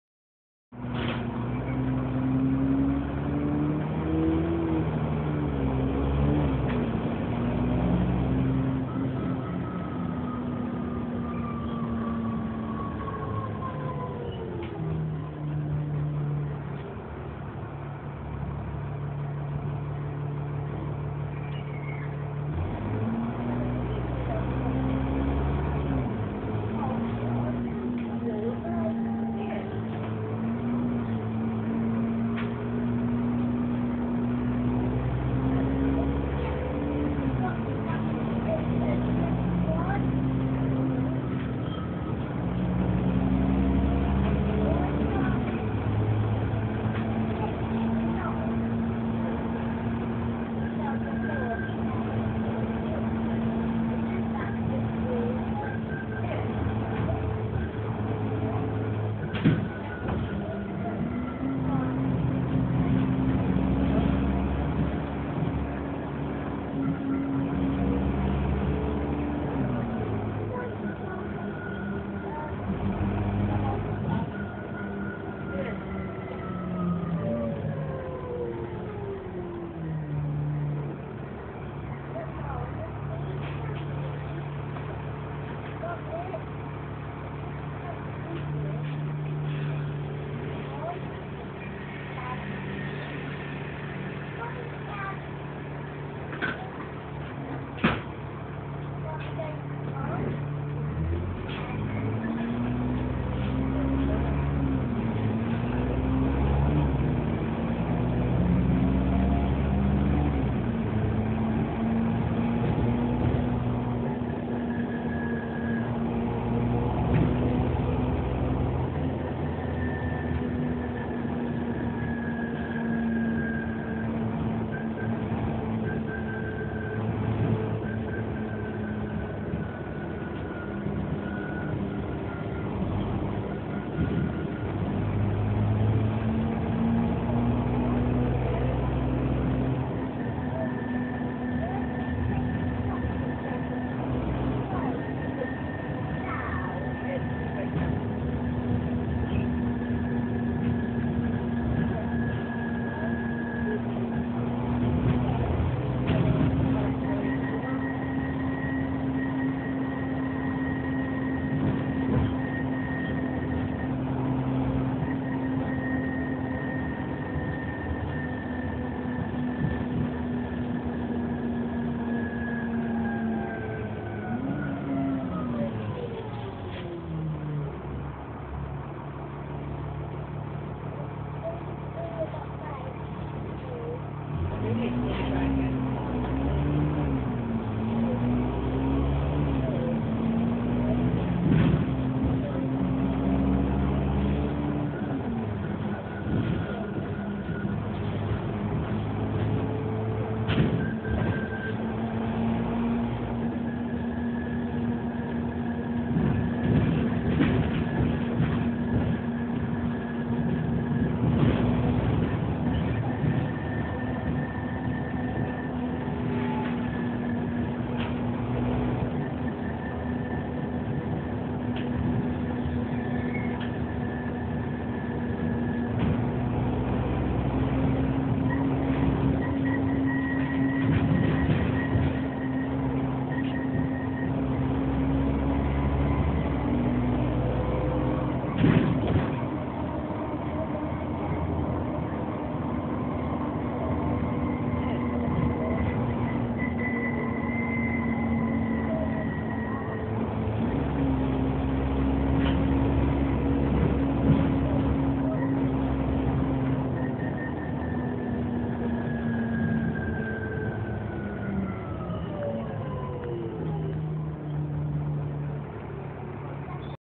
NXWM Alexander TransBus Trident 2 4429 BJ03ETX | Route 70 | 31/10/2013
National Express West Midlands TranBus Trident 4429 operating on the 10:50 70 Journey from Solihull to Birmingham filmed here from Solihull Town Centre. 4429 is currently based at Birmingham Central Garage